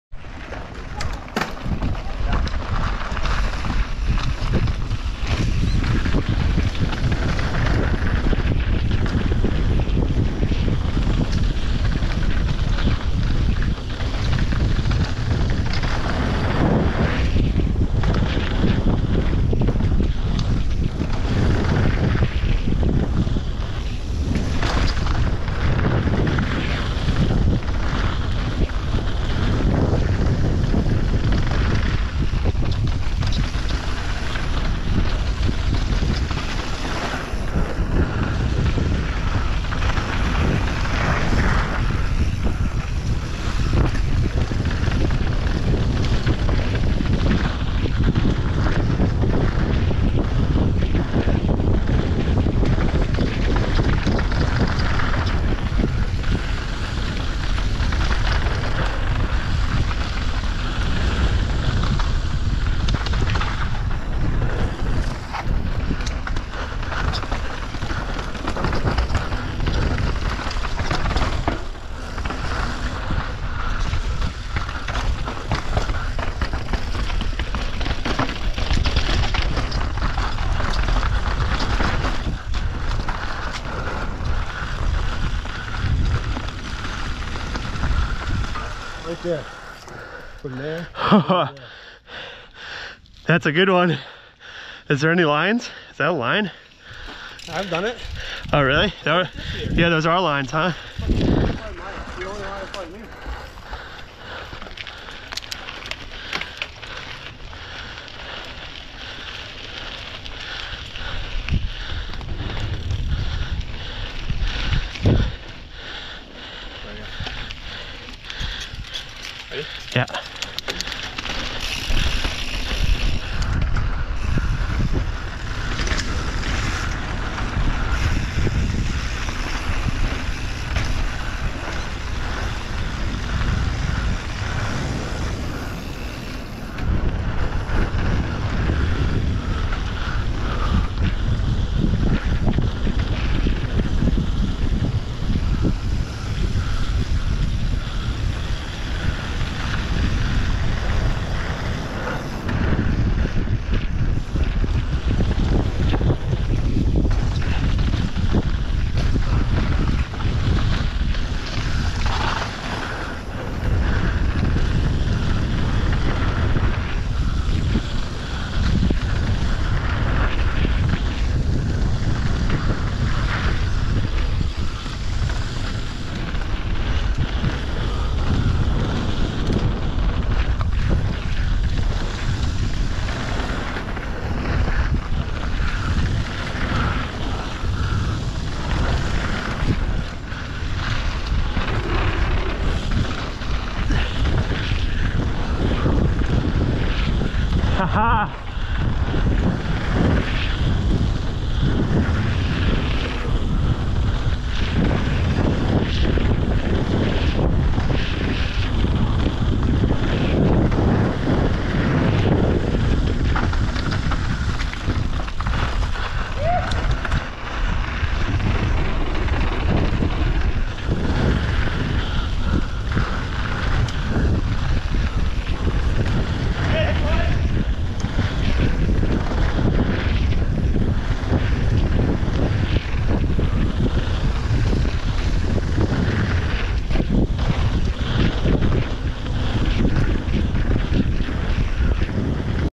BEST WARM UP LAP @HighlandMountainBikePark Cats Paw,FancyFeast,Lynx,Meadows End, Cat Scratch Fever
Meow Mix - Warm up run w/ Highlands Mountain Bike Park local Colton Hutton - Cat's Paw to Fancy to Lynx To Freedom to Meadows to Cat Scratch.